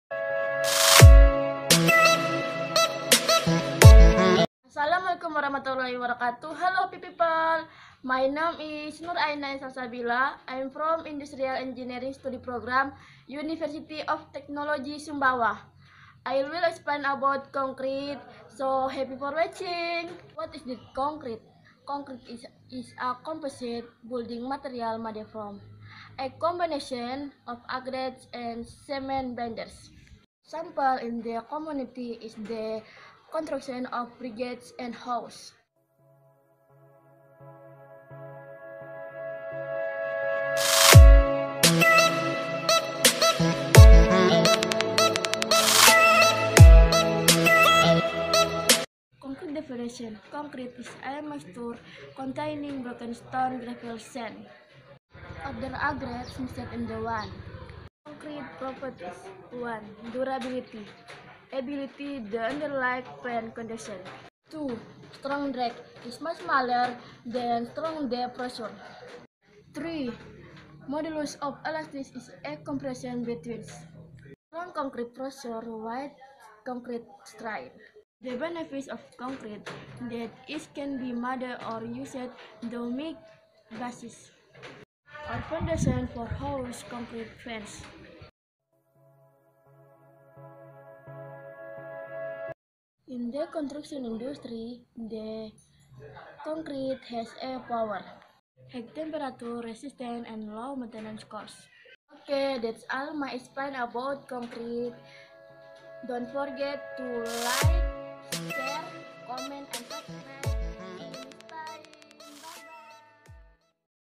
BETON? CONCRETE? - Noor Ainaya Salsabila
Video ini merupakan tugas pengganti Ujian Akhir Semester Mata Kuliah Pengetahuan Bahan Teknik oleh Noor Ainaya Salsabila, Teknik Industri 2018